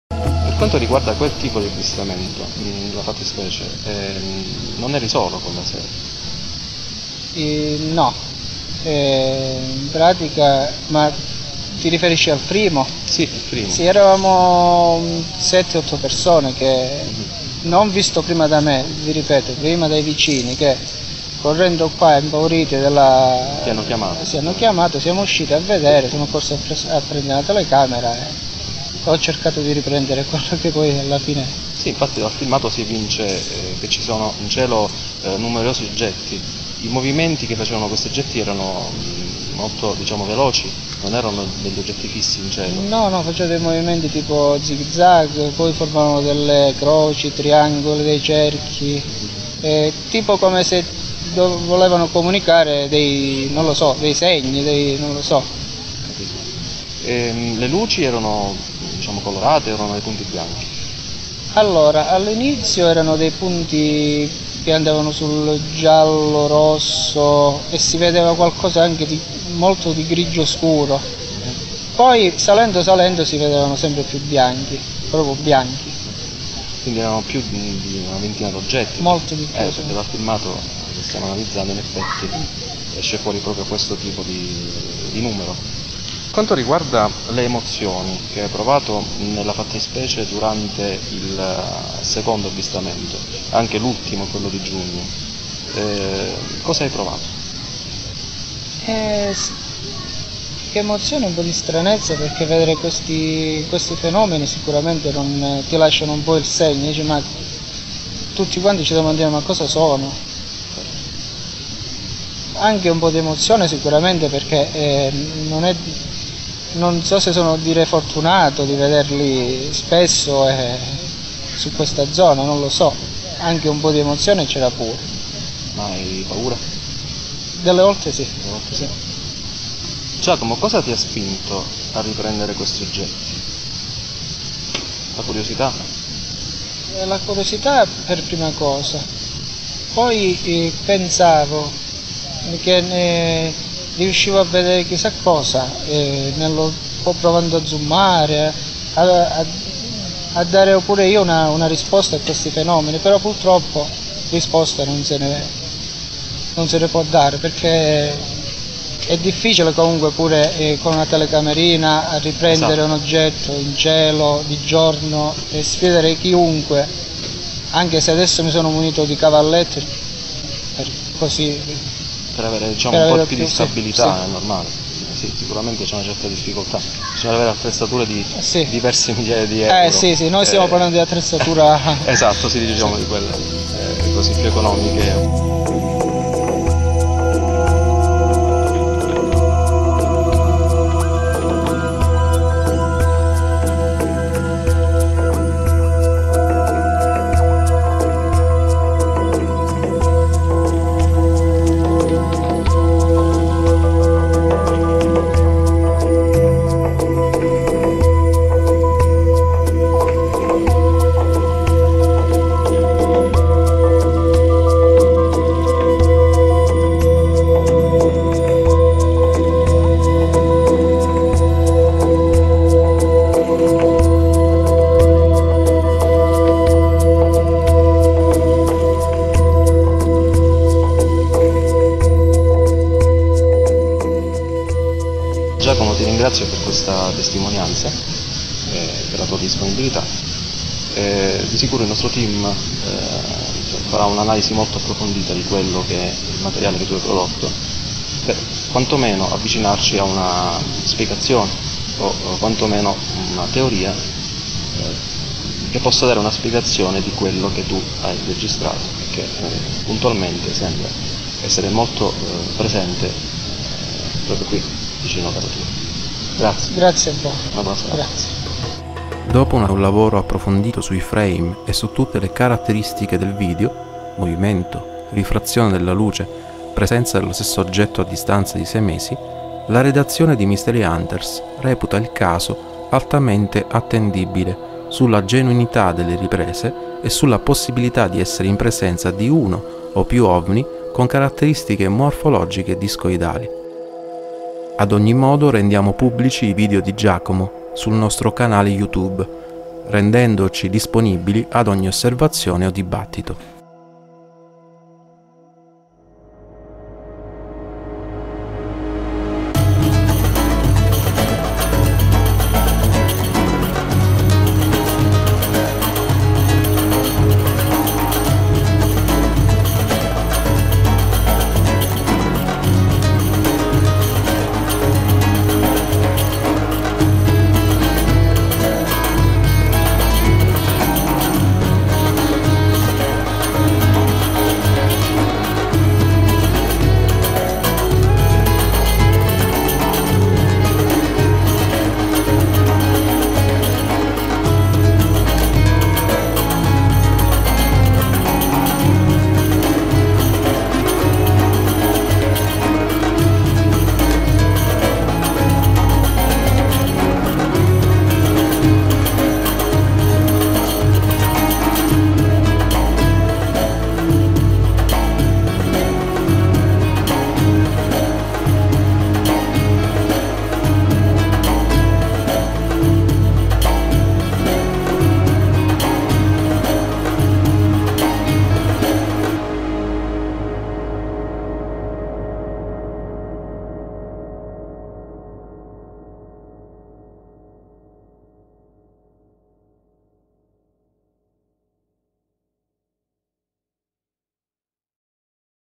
MISTERY HUNTERS - Ufo a Rende (Cs) 2/2
Presentiamo in esclusiva sul nostro canale youtube l'intervista al sig. Giacomo che ha ripreso nei cieli della zona industriale di Rende (Cs) quello che sembra essere un UFO in due momenti distinti il 21/01/2012 e il 24/06/2012. Ci parlerà anche di un avvistamento collettivo avvenuto nel 2010 di una flottillas volteggiante sopra la zona di residenza.
Ricordiamo che la zona in questione (c.da Lecco di Rende - Cs) è teatro da diversi anni di avvistamenti di Oggetti Volanti Non Identificati da parte dei residenti e dei lavoratori della zona.
Molti di questi casi sono già stati documentati con foto e testimonianze da parte del C.U.N. da diversi anni.

Il team dei Mistery Hunters vi presenta i filmati originali del sig. Giacomo nella loro interezza e anche un analisi approfondita dei frames più interessanti alla fine di ogni ripresa cliccando sul seguente